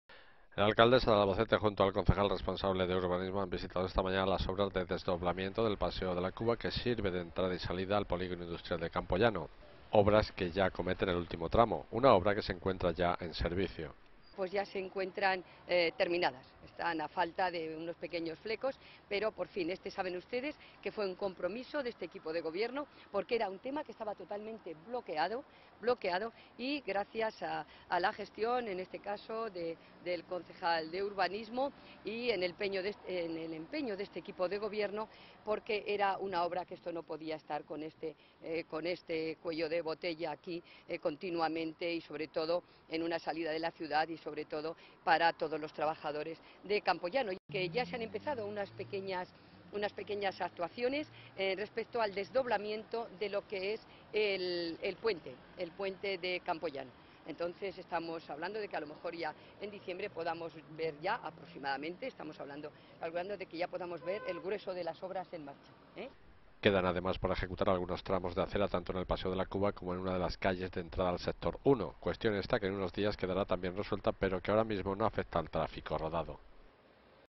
Visita a las obras del desdoblamiento del paseo de la Cuba
LA ALCALDESA DE ALBACETE JUNTO AL CONCEJAL RESPONSABLE DE URBANISMO HAN VISITA ESTA MAÑANA LAS OBRAS DE DESDOBLAMIENTO DEL PASEO DE LA CUBA QUE SIRVE DE ENTRADA Y SALIDA AL POLIGONO INDUSTRIAL DE CAMPOLLANO. OBRAS QUE YA ACOMETEN EL ULTIMO TRAMO, UNA OBRA QUE SE ENCUENTRA YA EN SERVICIO